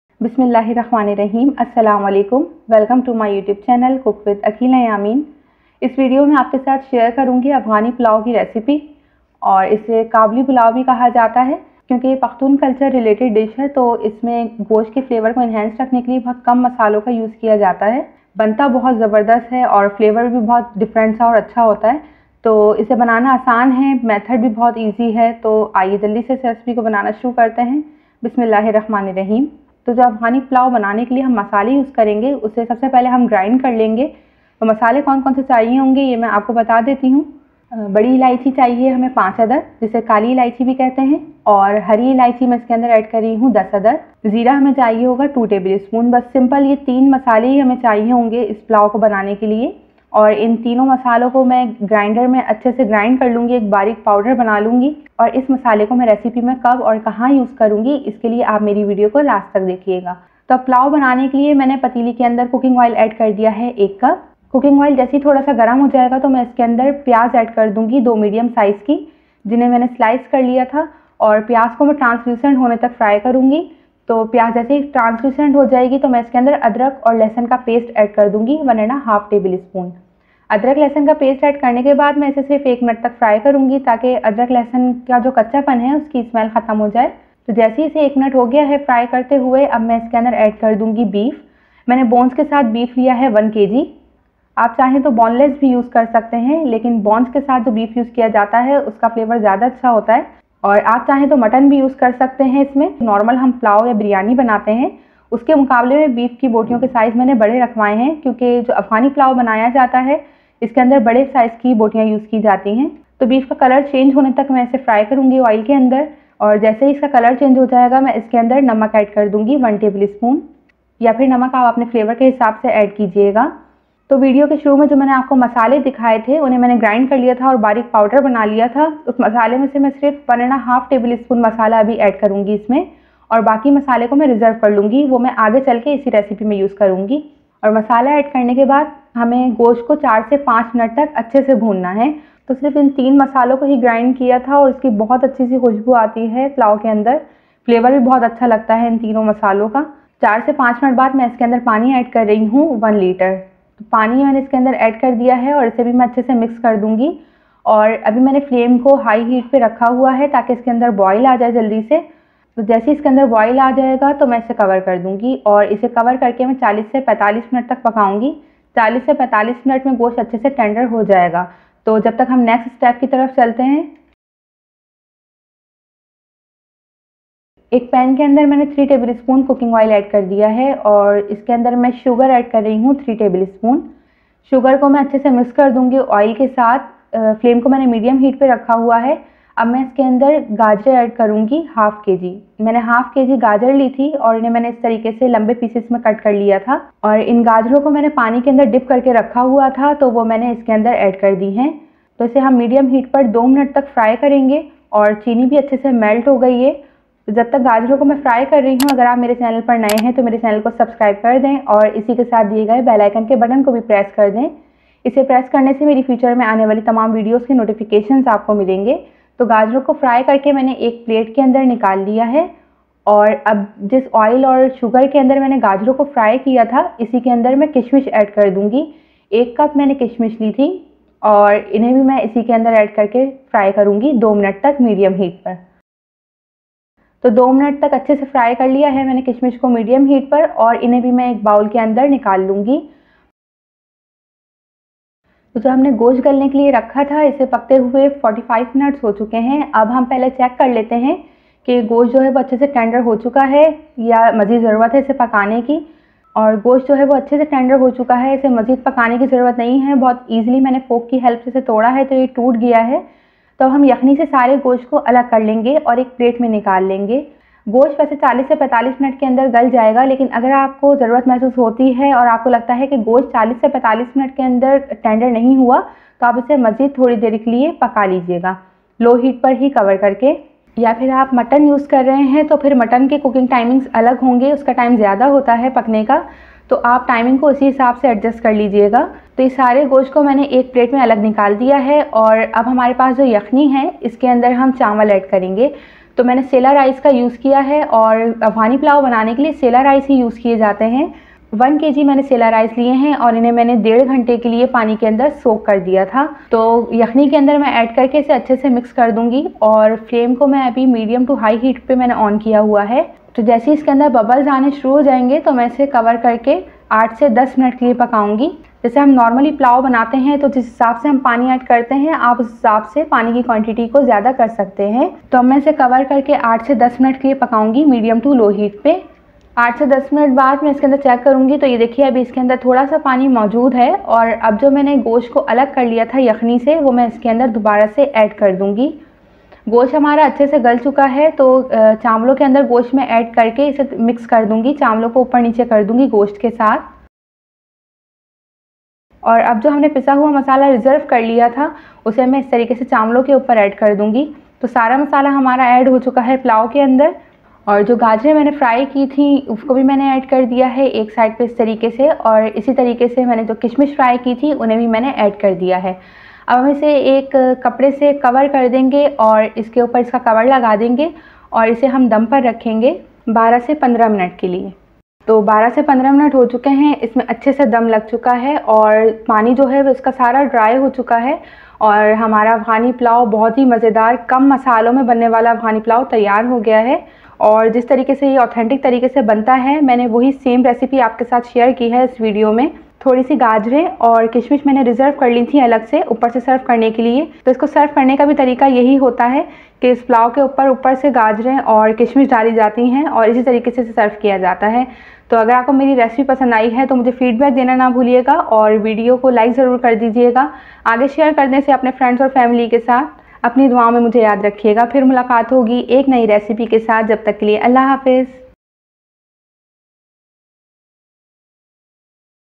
Kabuli pulao recipe | Afghani Pulao Recipe | Beef Afghani Pulao | Uzbaik Pulao | Beef Yakhni pulao
Cook with Aqeela Yameen
Delisious recipes
In this video i am going to make afghani pulao recipe
this is authentic and perfect recipe
also called kabuli pulao recipe
Bakra eid / eid ul azha special recipe
Uzbaik Pulao recipe